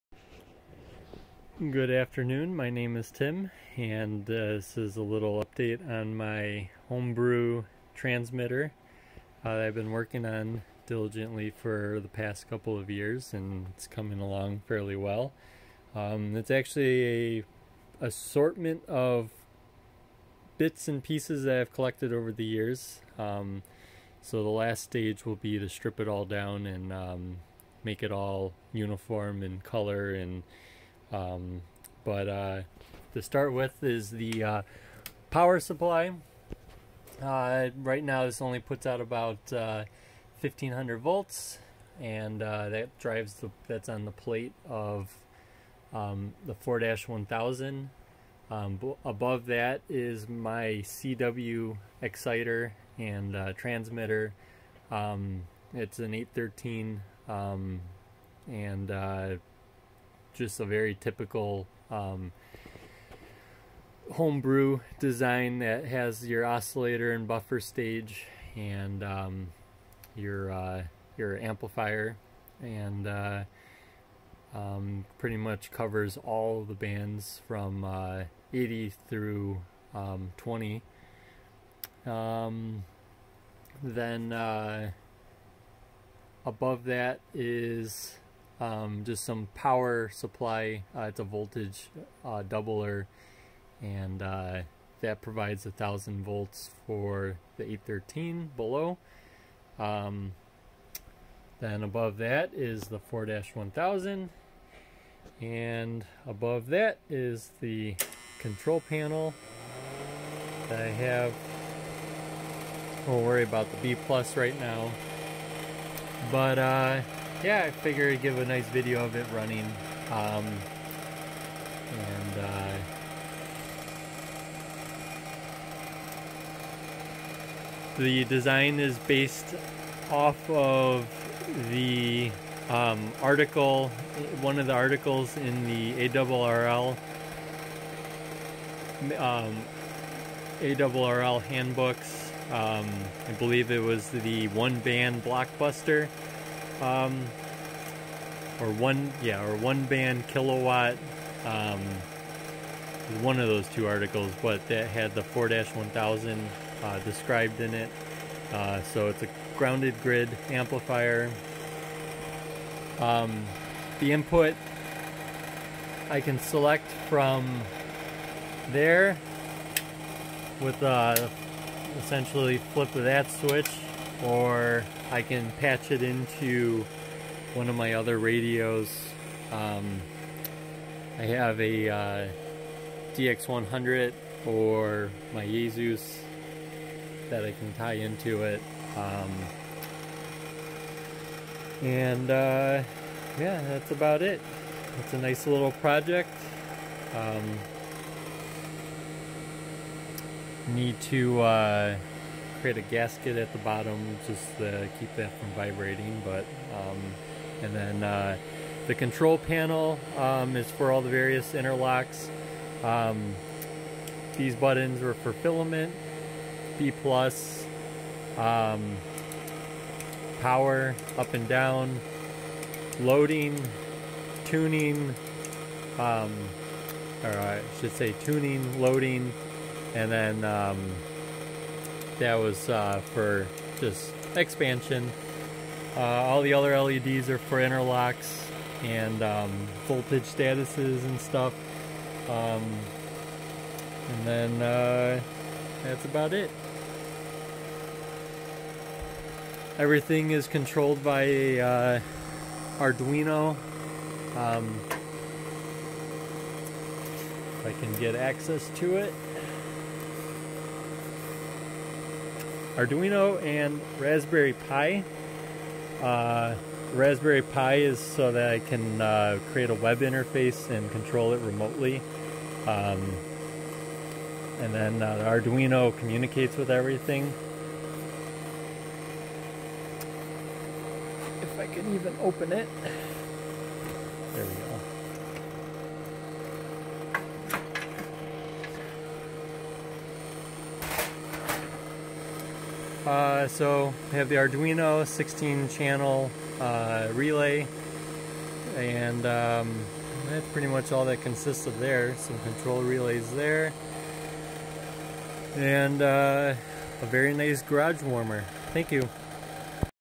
1983 Handbook 4-1000a Tube Transmitter
Amateur Radio Handbook 1983 one band kilowatt. Grounded grid class c amplifier.